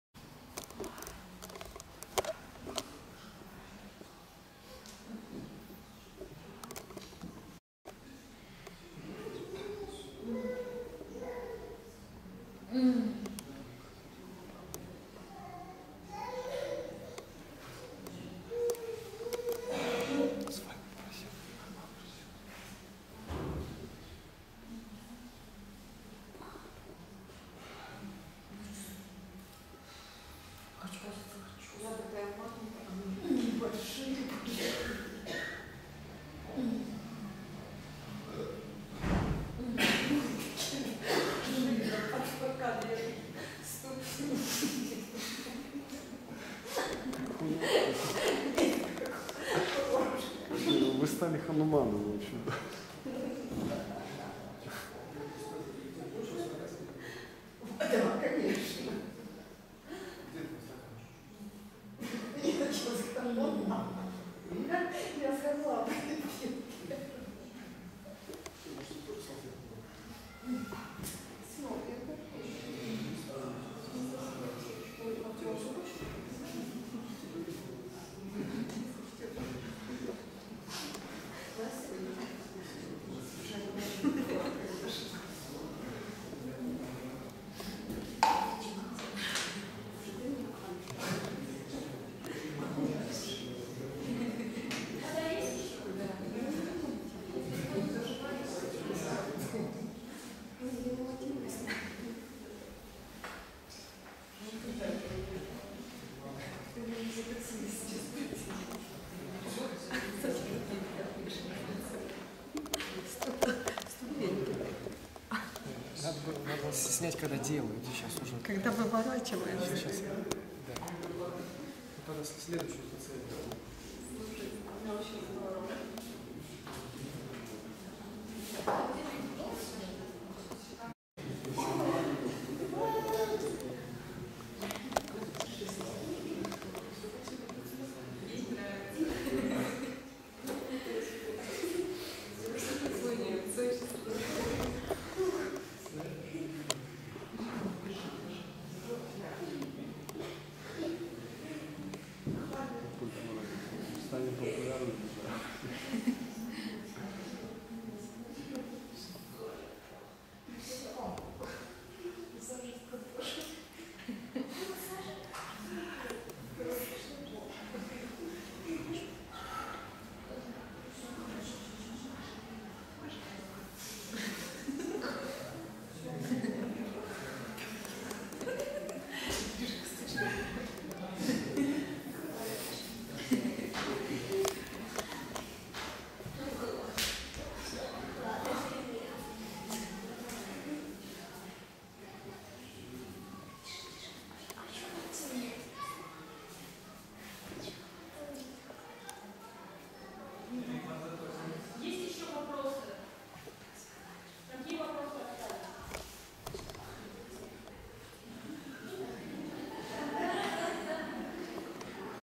2014 08 27 Acupressura i Marma massag 3 den Ucheniki
На встрече 27 августа в Троицком Научном центре Свами рассказывает об акупрессуре и марма массаже. А также проводит диагностику. Отвечает на вопросы.